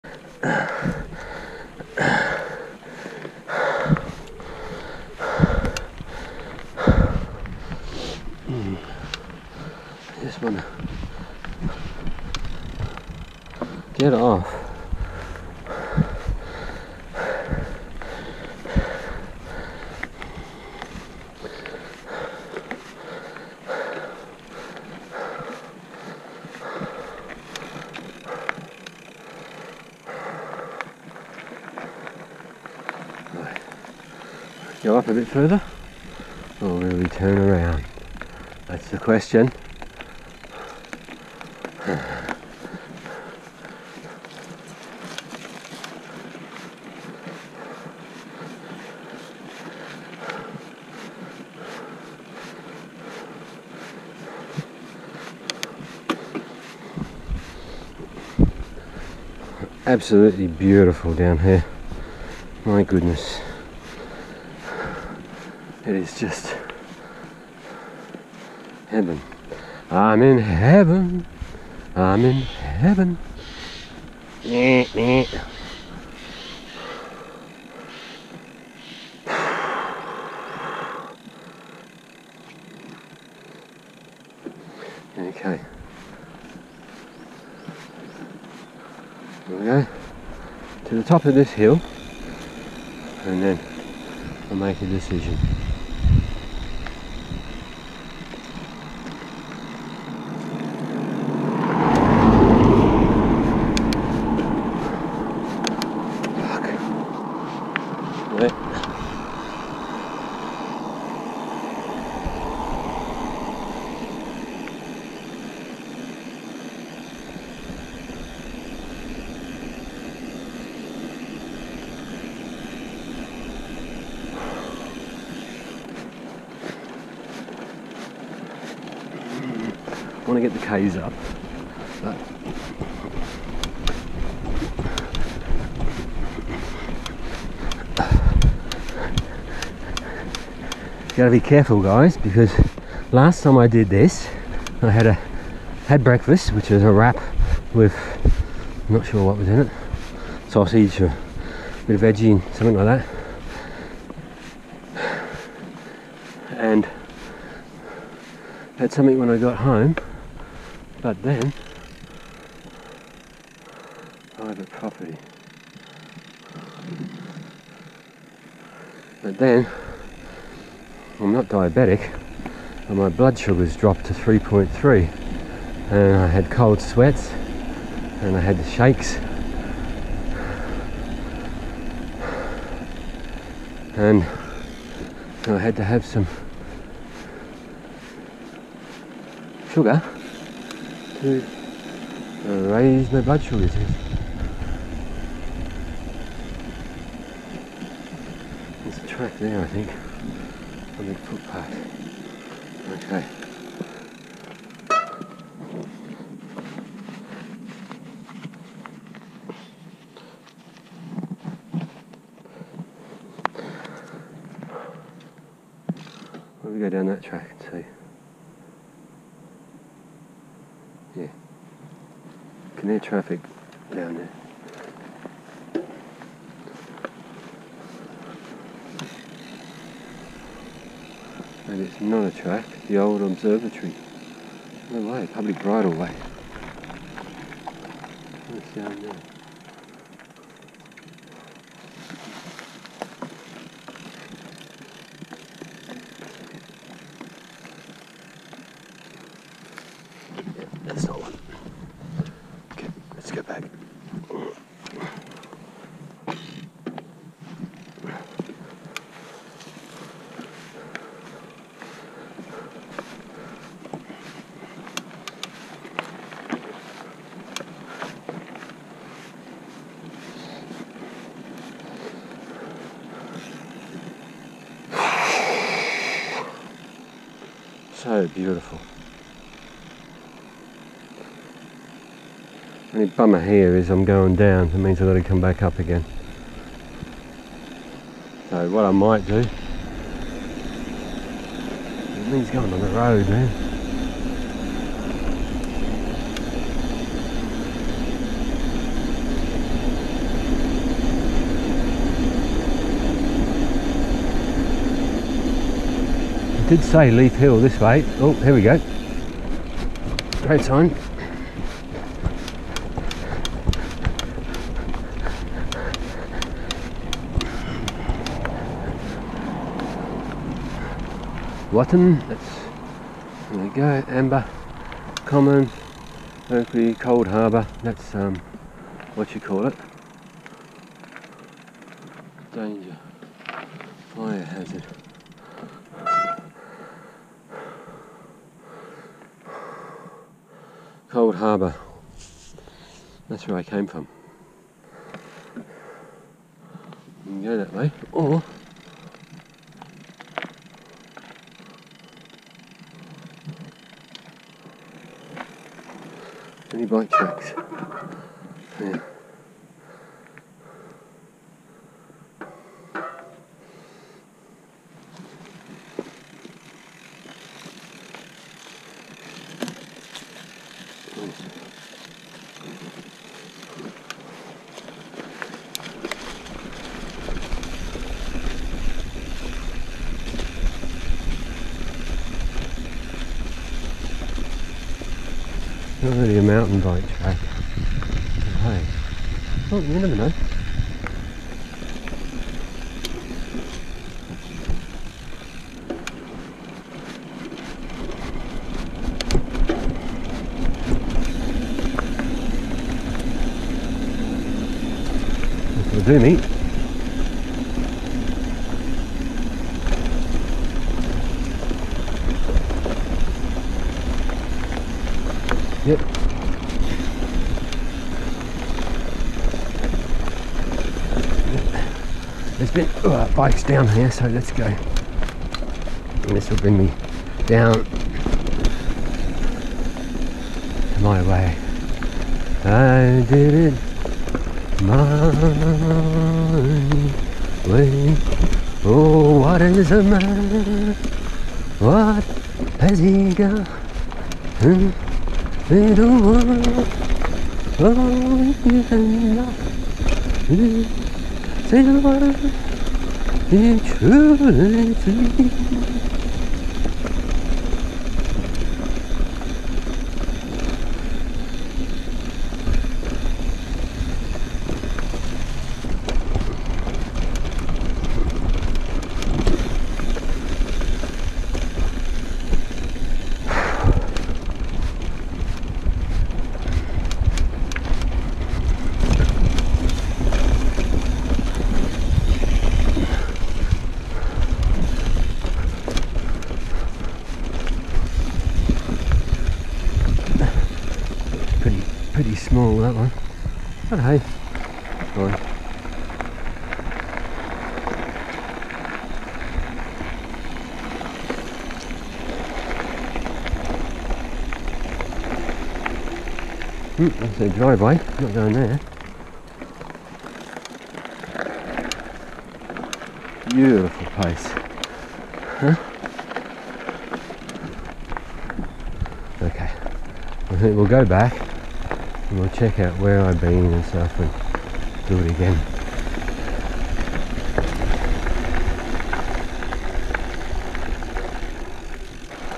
Surrey Hills. No MTB trails just a nice summers day cycle in Beautiful Surrey Hills.
Nothing special just a nice cycle during the English summer through the Surrey Hills, excuse my singing.
Keep Cycling & Get Happy.

Filmed using my GoPro Hero5 session.